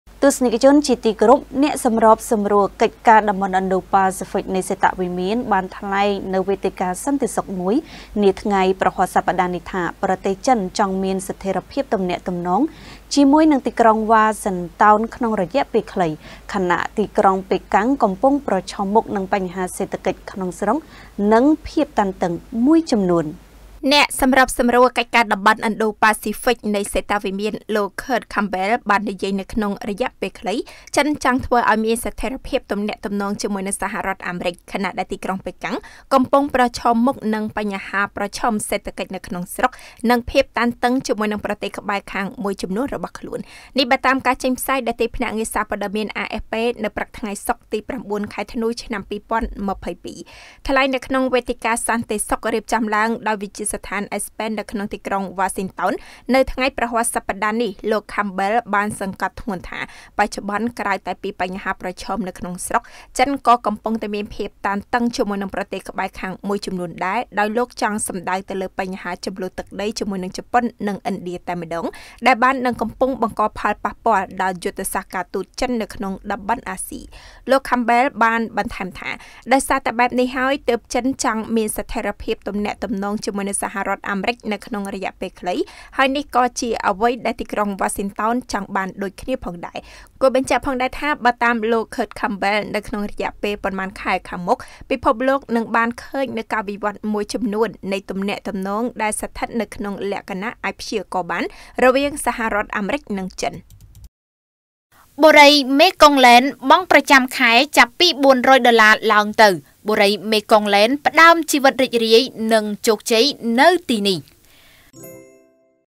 សហរដ្ឋអាមេរិក ថាចិនចង់មានស្ថិរភាពទំនាក់ទំនងជាមួយវ៉ាស៊ីនតោន ក្នុងរយៈពេលខ្លី
* រាយការណ៍ដោយ កញ្ញា យឹម ចាន់ស្រីវីន  និងកញ្ញា យ៉ង់ សក្គុណា